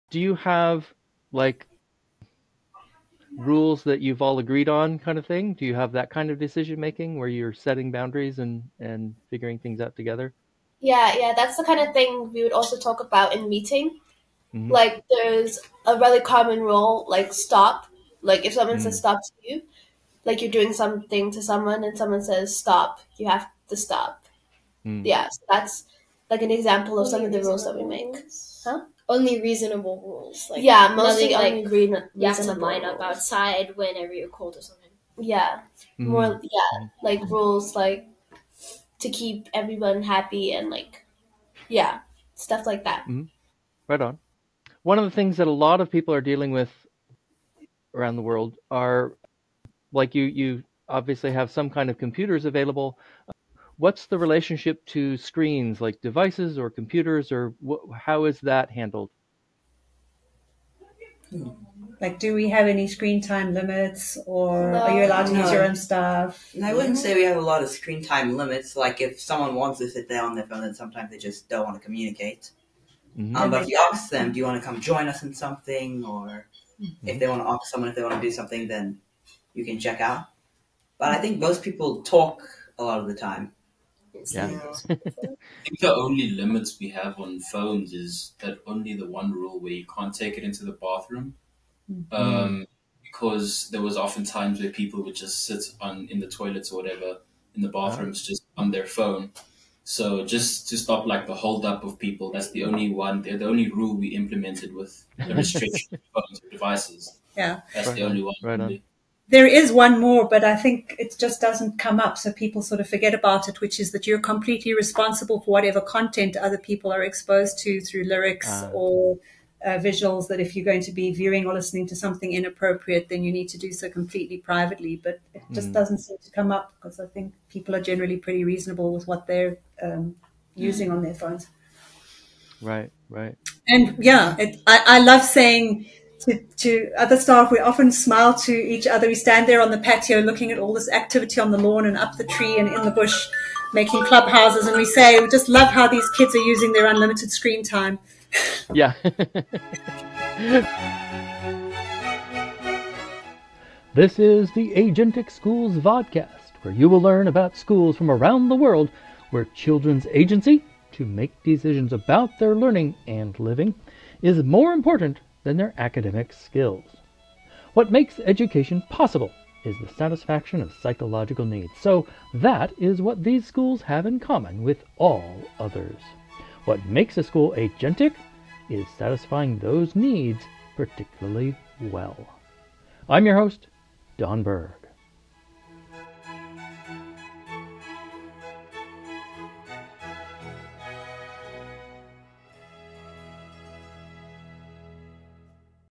How students use unlimteed screentime- Jeanna Clements - Riverstone Village - S2E13x5
Guests: Je’anna Clements, Bonolo, Angelo, Ruqayyah, Hassan, & Kate - Riverstone Village, Randburg, Johannesburg, South Africa